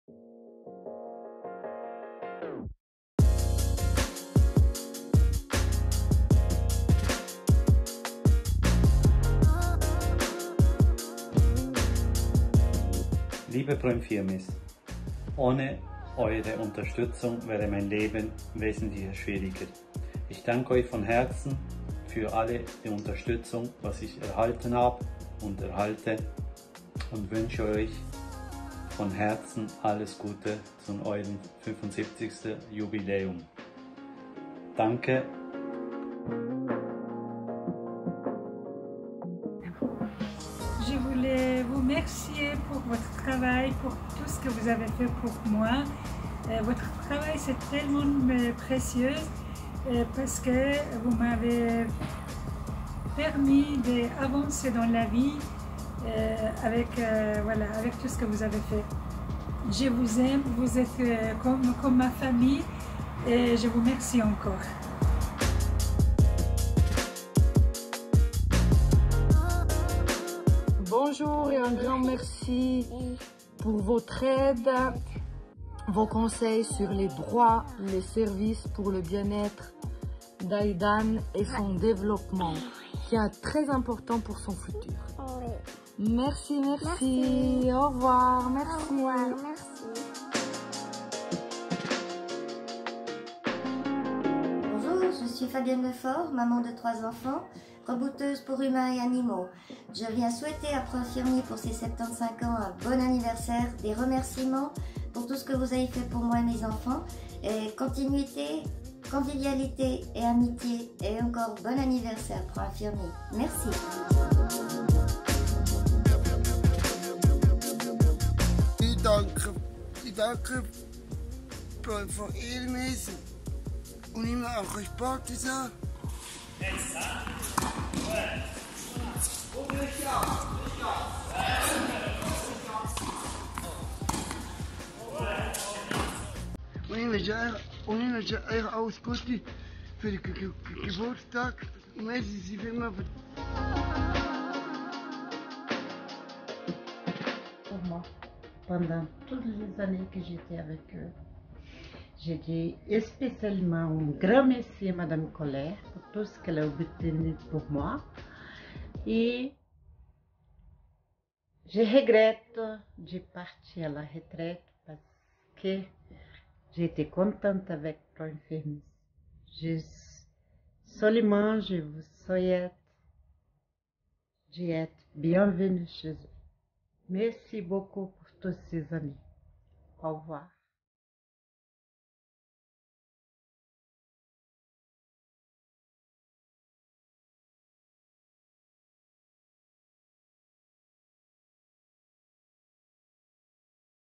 Les client·e·s de Pro Infirmis Fribourg s’associent à la fête !
Par cette vidéo, nos bénéficiaires nous adressent leurs vœux d’anniversaire et remercient le combat mené par Pro Infirmis Fribourg pour l'inclusion depuis 75 ans.

Le slogan « l'avenir ne connaît pas d’obstacle », que nous vous ferons découvrir au cours de cette année, a l’ambition de mettre en avant le potentiel et les richesses des personnes en situation de handicap, afin d'encourager leur inclusion dans la société.

« Je voulais vous remercier pour votre travail pour tout ce que vous avez fait pour moi. Votre travail est tellement précieux parce que vous m’avez permis d’avancer dans la vie avec tout ce que vous avez fait. » Une cliente de Pro Infirmis Fribourg.

Pour en savoir plus: proinfirmis.ch/75ans